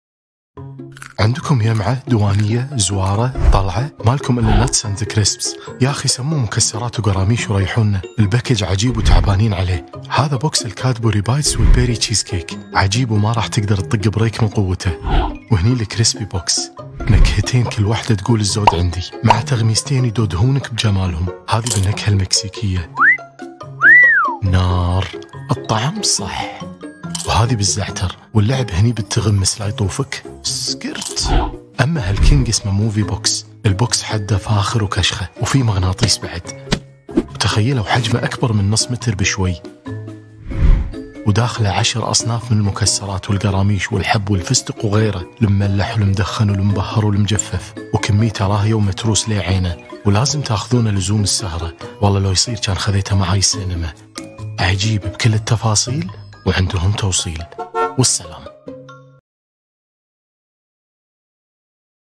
nuts.crisps | ARABIC snacks | Try it and be healthy Of course you can eat it | Arabic food
nuts.crisps

Try it and be healthy Of course you can eat it  And if you tried it, take a picture and ask us to mention

Their location:

A central kitchen

Delivery:

Via the Whatsapp on their account

Note:

This is our personal opinion and it remains a matter of different tastes, so if you tried it, write us your opinion and let us benefit from your experience